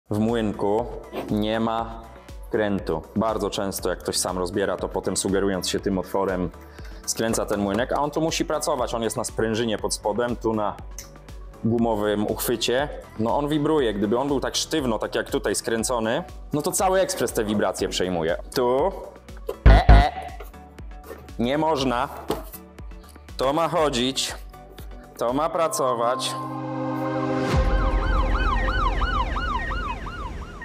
Saeco Incanto po własnoręcznym dłubaniu
Błędy przy montażu przez "fachowca"
Saeco Incanto HD8819/59

Cały film już w sobotę 22 kwietnia 2023 - godzina 16:00

Nowa kamera - Panasonic Lumix G7 12-60mm